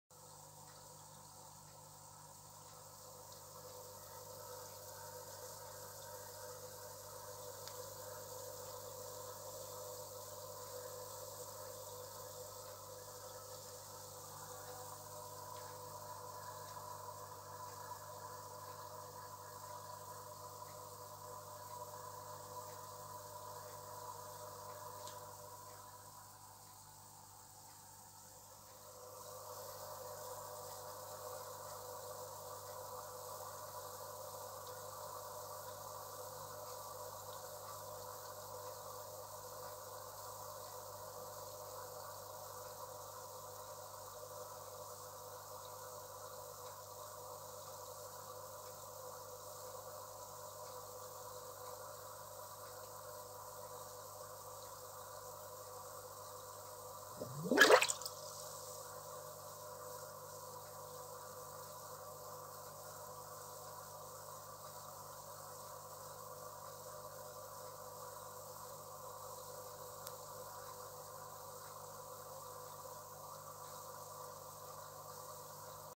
Natural breeding try-out for catfish
Trying out my own design of natural breeding tank for catfish. Hoping the catfish likes it and breed in it.